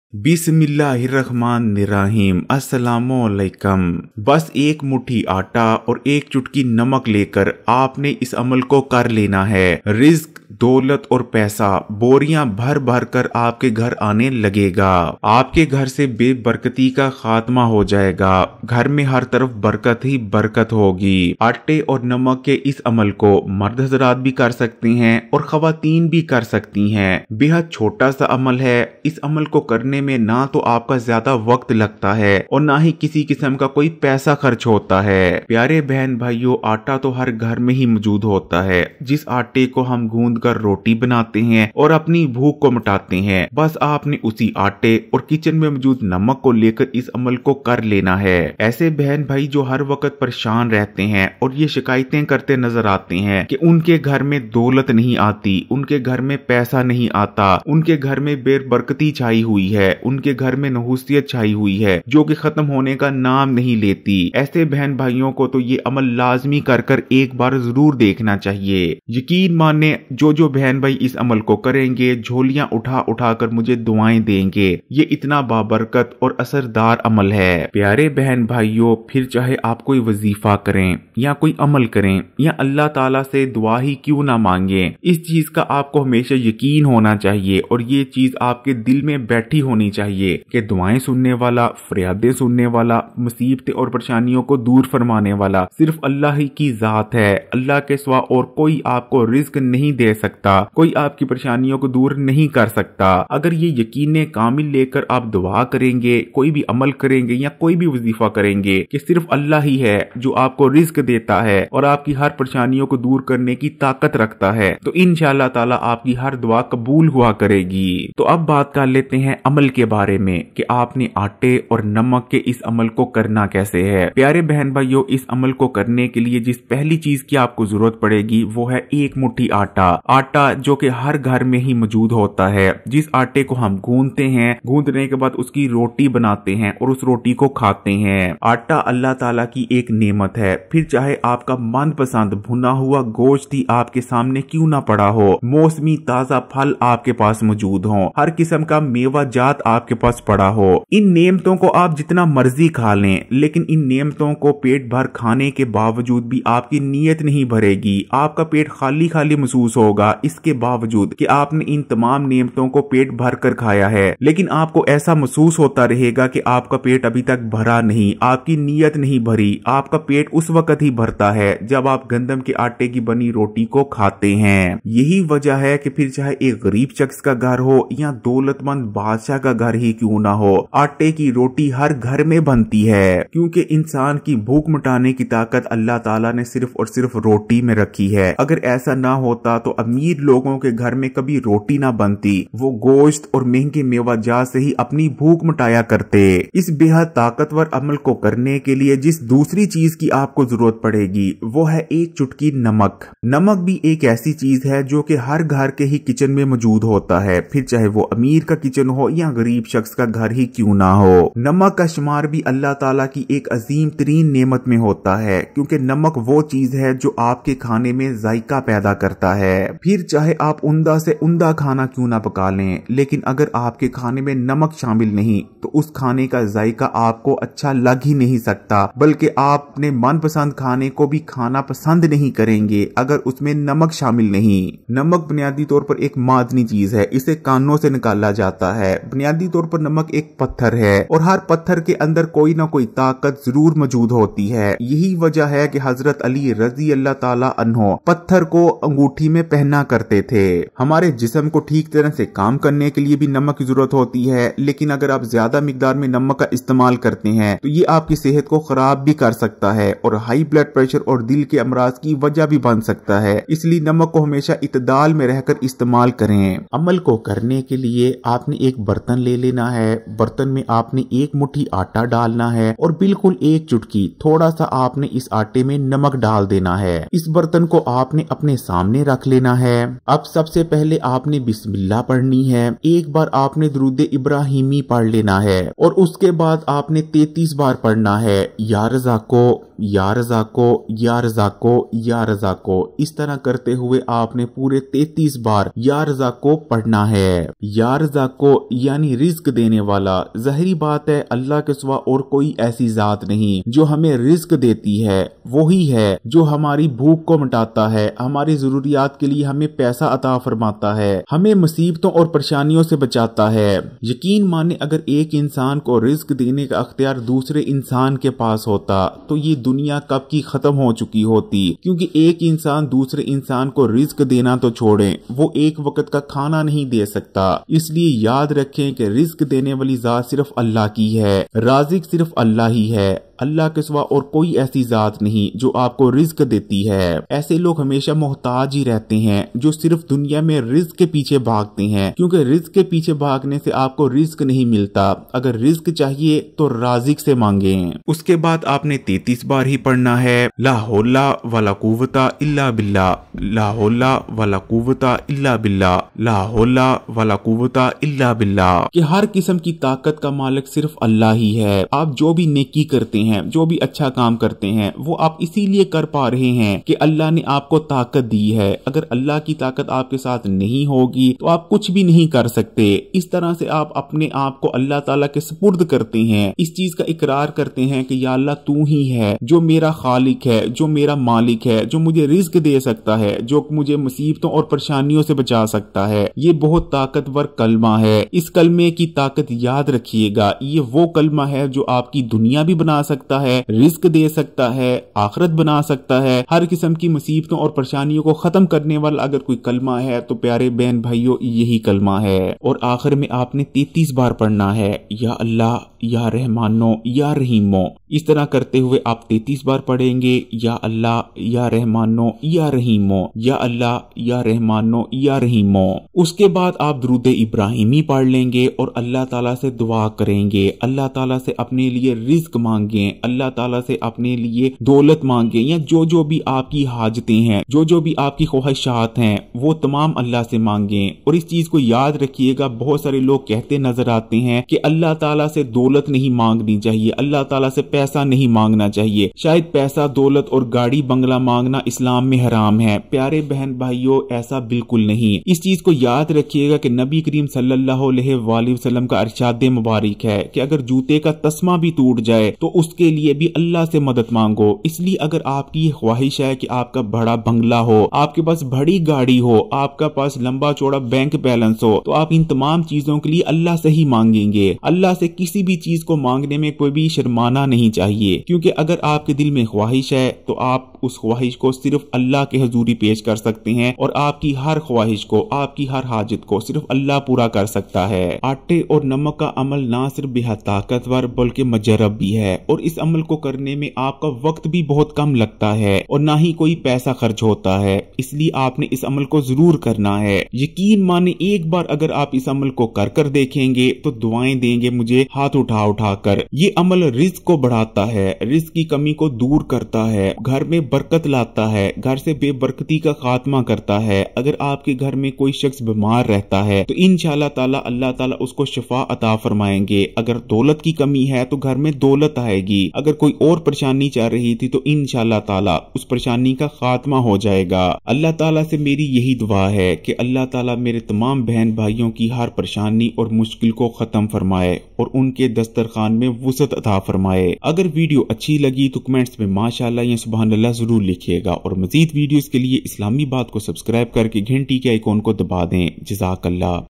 Ek Muthi Namak aur Atta | Dolat Rizq Aur Barkat
Bas app ne ek muthi atta aur ek chotki namak le kar ye amal kar lene hai, phir deken kase rizq, dolat ki barish hoti hai, app ke ghar see be barkati ka khatma ho jae ga aur ghar mein barkat aye gi.

You just need a handful of flour and a pinch of salt for this unique Amal. This can change your life, make you rich and successful. All of your problems will be solved. You will be blessed with money and happiness.